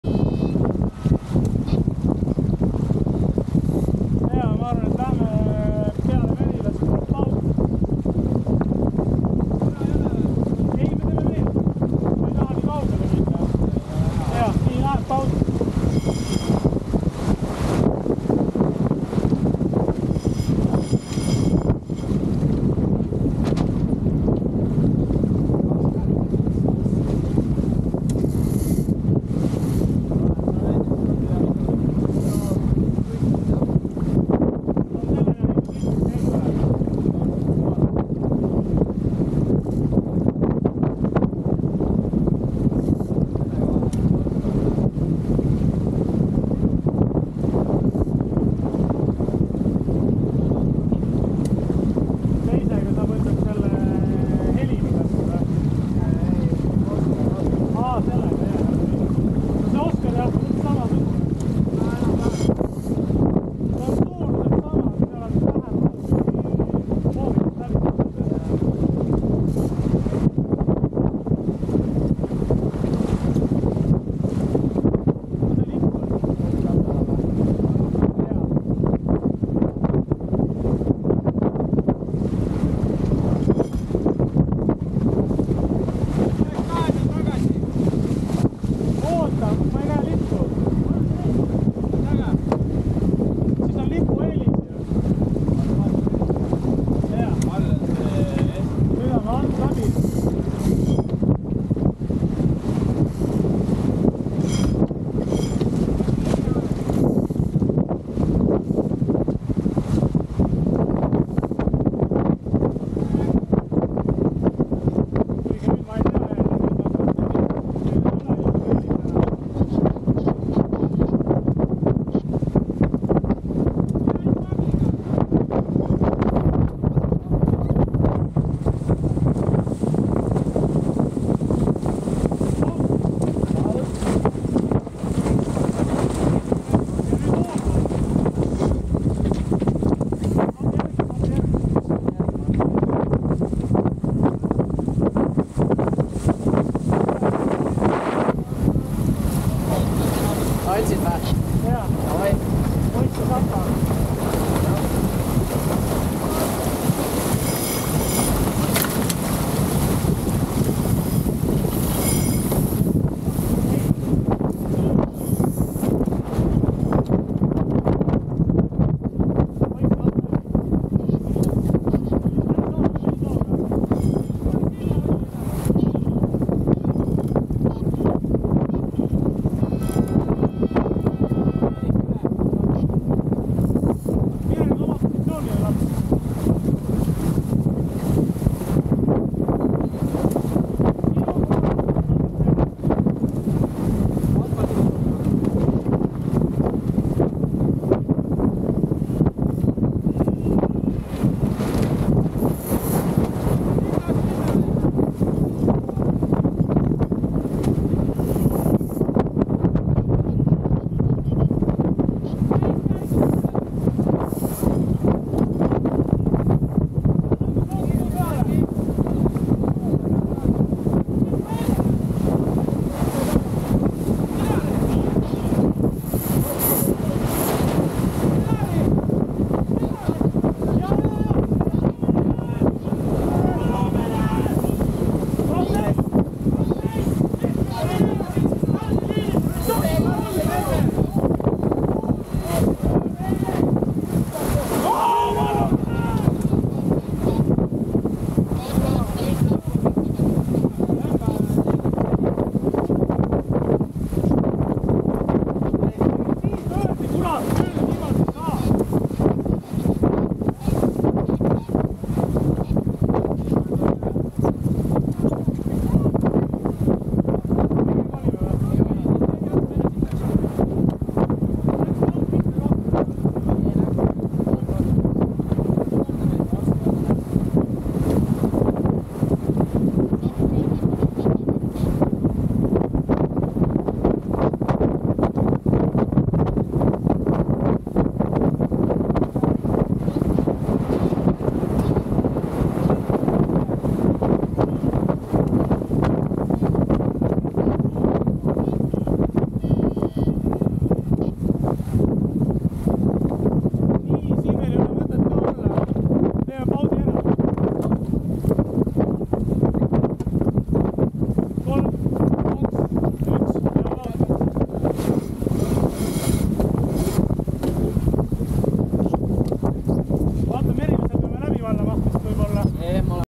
Small crash at Wednesday night sailing race start: KJK kolmapäevak 27.09.2023.
Video starts with 4 minute signal
X79 and First 31.7 not keeping clear